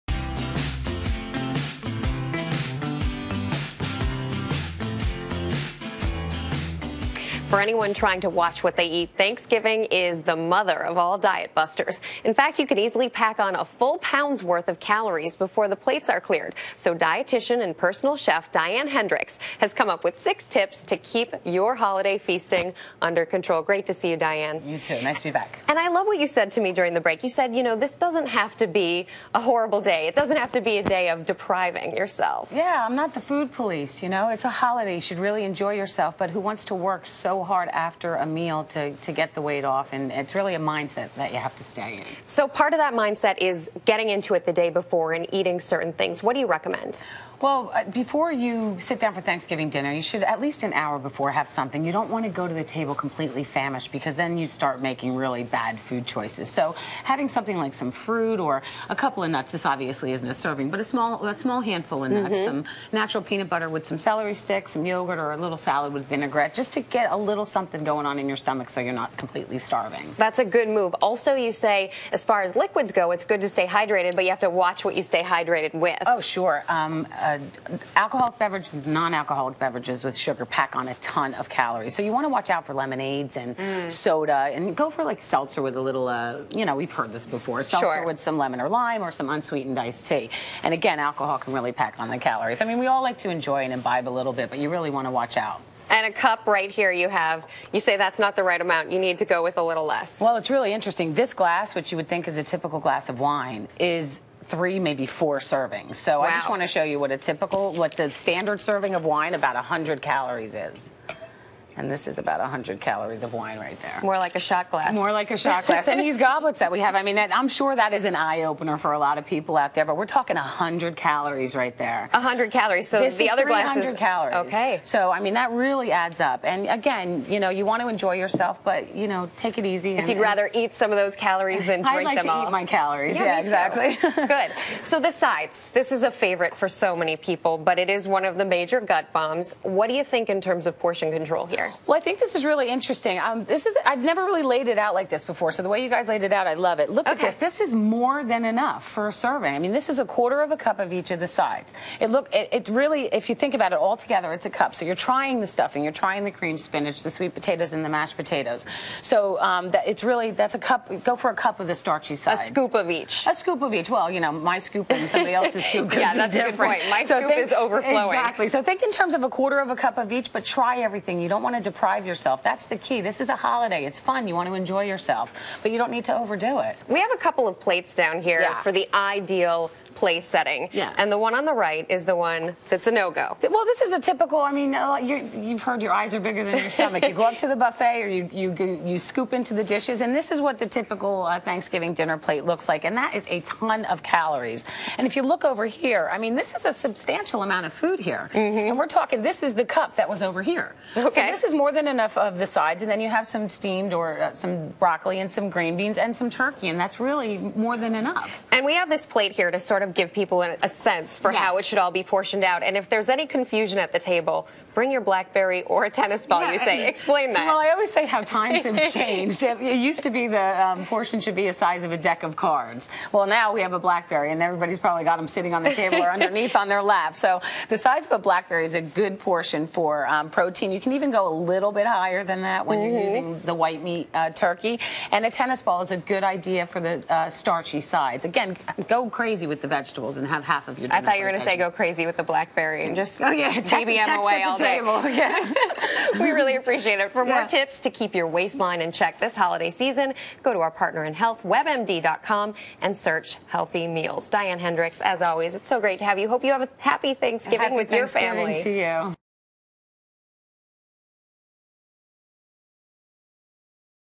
Controlling Thanksgiving Portion Sizes
It's not easy to make good decisions at the table when you're surrounded by so many once-a-year, favorite dishes but dietitian and personal chef Diane Henderiks shared some great tips with Rebecca Jarvis to keep you from doing damage to your waistline over Thanksgiving.